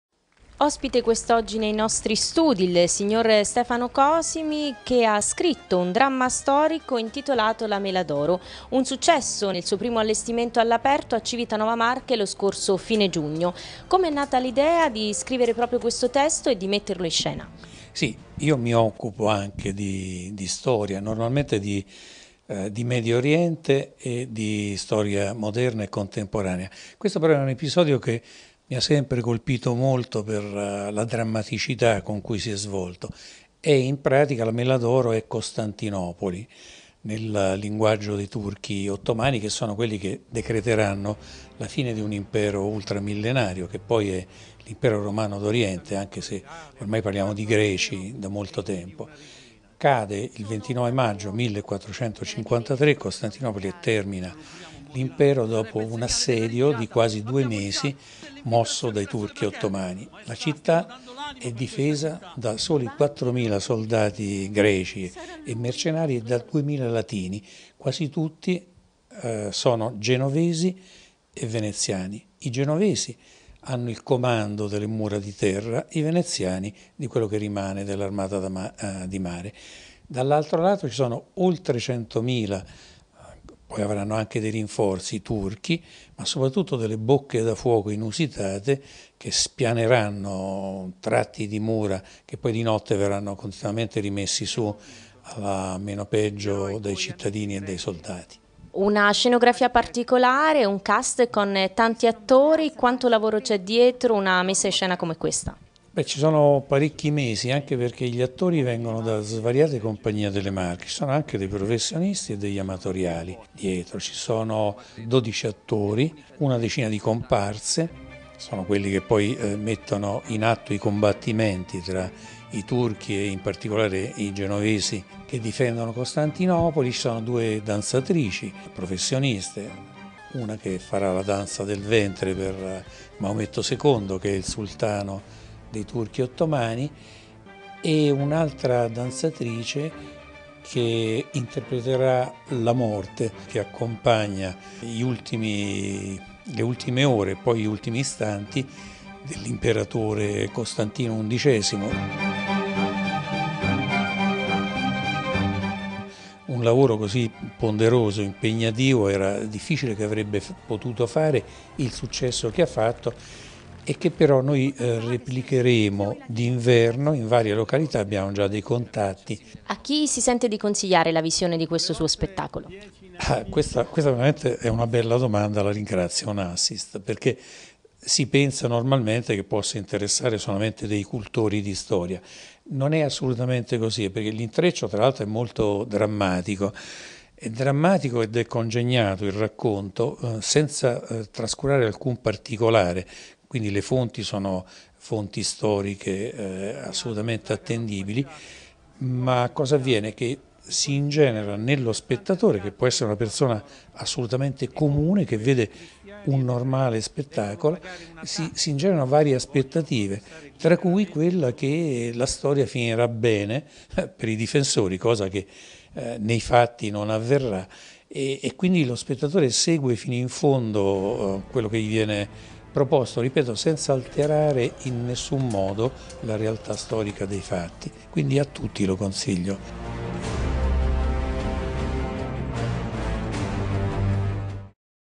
"La mela d'oro", il dramma storico scritto da Stefano Cosimi
Successo inaspettato nella sua prima messa in scena all'aperto a Civitanova Marche per il dramma storico "La mela d'oro - La caduta di Costantinopoli" scritto da Stefano Cosimi, che abbiamo avuto il piacere di intervistare nei nostri studi